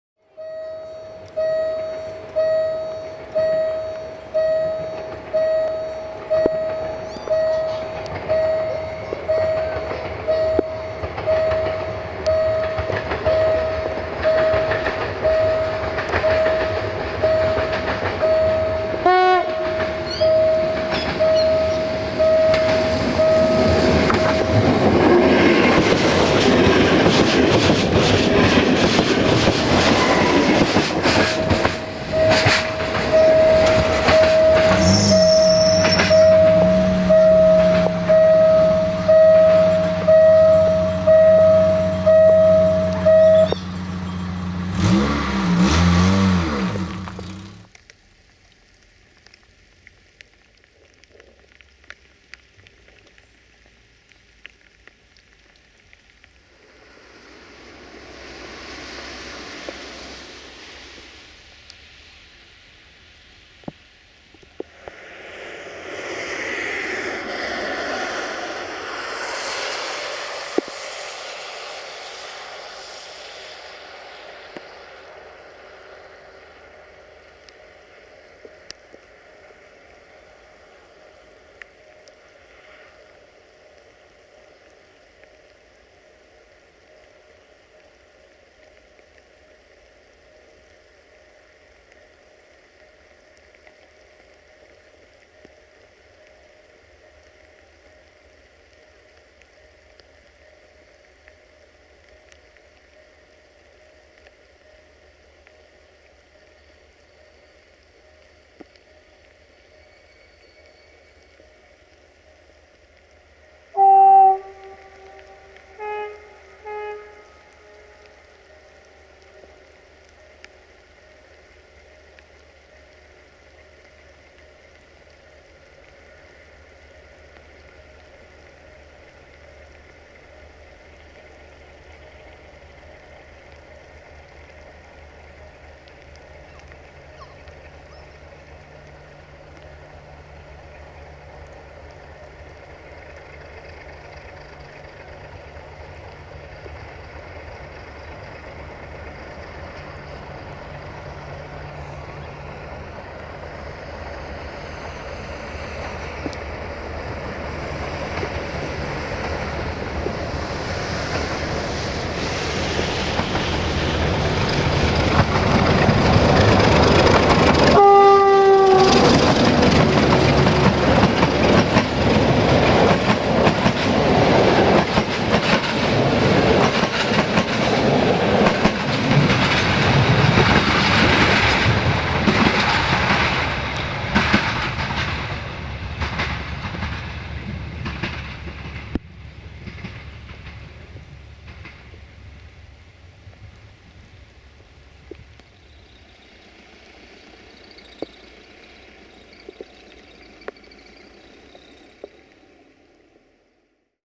VT614 DMU, 060-DA1 62-1051-2 & LDH 82-0561-9 in Apuseni Mountains [Rain Edition] - 22 November 2015
1:IR15206 - Oradea - Cluj Napoca - TFC - Automotor VT614 DMU/Diesel Triebwagen
2:R3076 - Oradea - Cluj Napoca - 060-DA1/ST43 - LDE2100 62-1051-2 RO-SNTFC & LDH1500 - Torpila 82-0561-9 RO-SNTFC

22 November 2015

© Train97